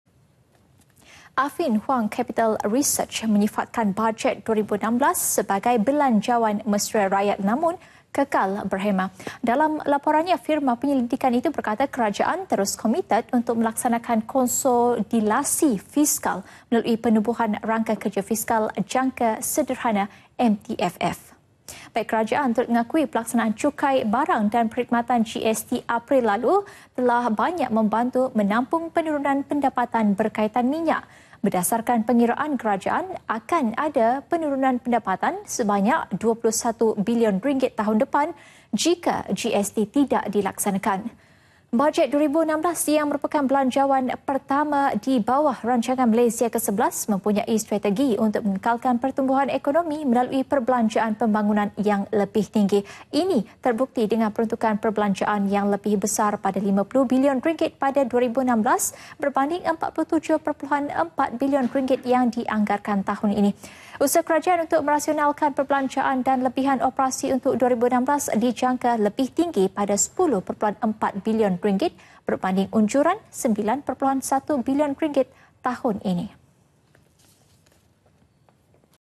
BAJET 2016 MESRA RAKYAT NAMUN KEKAL BERHEMAH [26 OKT 2015]
Affin Hwang Capital Research menyifatkan bajet 2016 sebagai belanjawan mesra rakyat; namun kekal berhemah.

Dalam laporannya, firma penyelidikan itu berkata kerajaan terus komited untuk melaksanakan konsolidasi fiskal melalui penubuhan Rangka Kerja Fiskal Jangka Sederhana, MTFF.